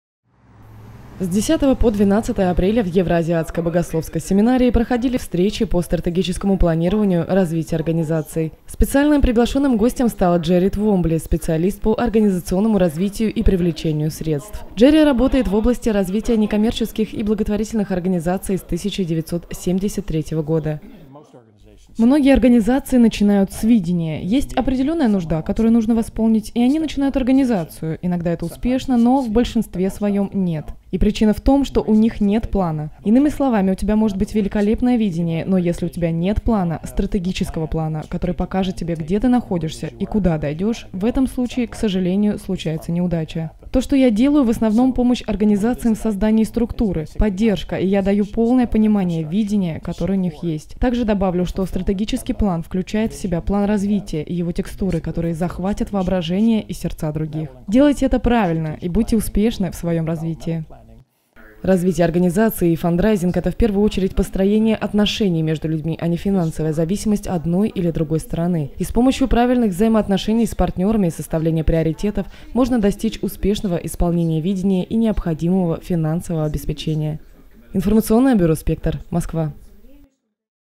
Джерри Твомбли в Евроазиатской богословской семинарии
С 10 по 12 апреля в Евроазиатской богословской семинарии проходили встречи по стратегическому планированию развития организаций. Специальным приглашенным гостем стал Джерри Твомбли — специалист по организационному развитию и привлечению средств (фандрайзингу). Джерри работает в области развития некоммерческих и благотворительных организаций с 1973 г. Это не первый его визит в Россию, в 2006 г. он проводил семинар для церквей и НКО по фандрайзингу, где он делился сорокалетним опытом успешного сбора средств для религиозных и некоммерческих организаций. В этот раз по приглашению Евроазиатской богословской семинарии Джерри Твомбли помогал работникам составить план развития до 2030 г.  «Процесс стратегического планирования включает в себя несколько этапов. Сначала нужно четко сформулировать миссию, которая будет раскрывать смысл деятельности организации, а затем проработать видение на несколько лет вперед», — рассказывал гость.